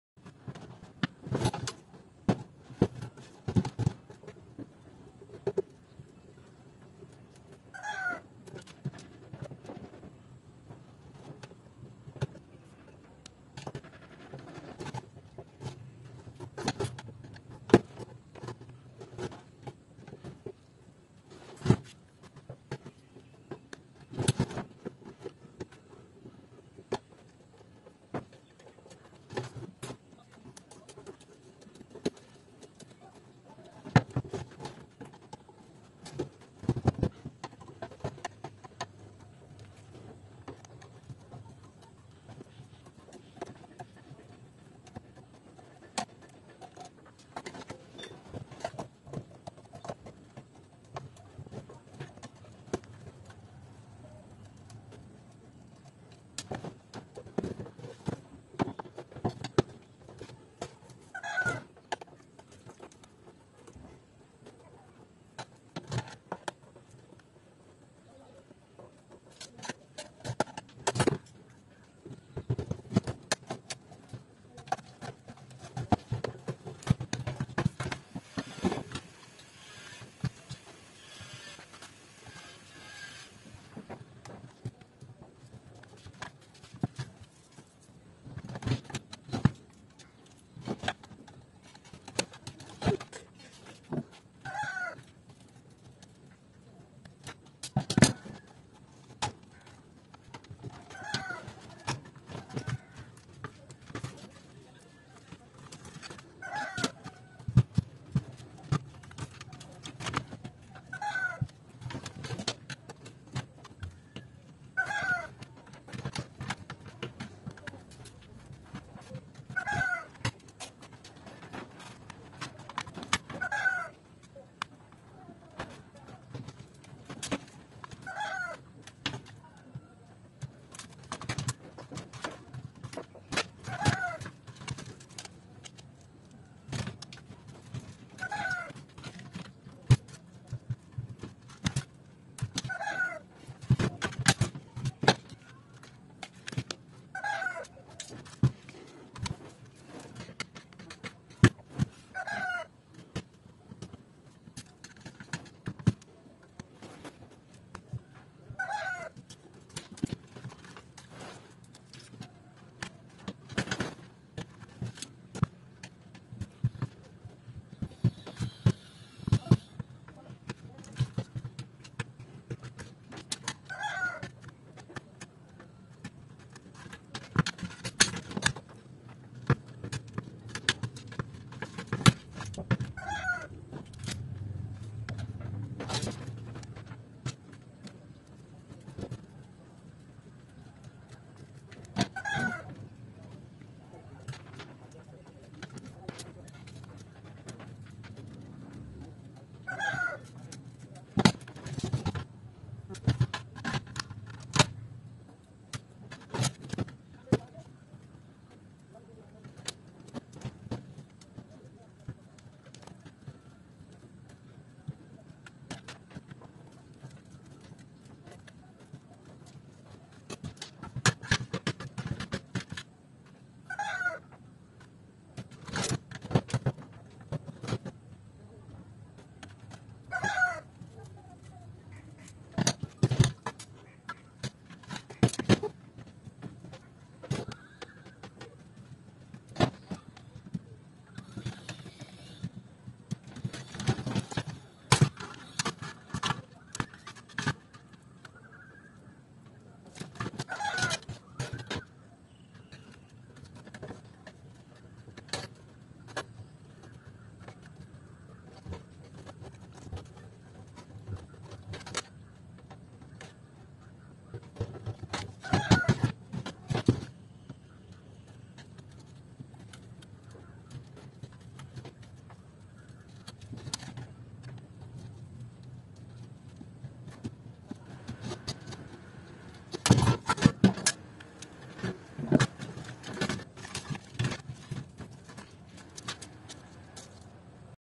What is inside DVD Player ?🤔 I opened the DVD Player.😱💀👽️😈👿🤖
A DVD player is composed of sophisticated electronics, including a disk drive mechanism, a printed circuit board, and an optical system assembly. It consists of a spindle that holds the disk and a motor that spins it.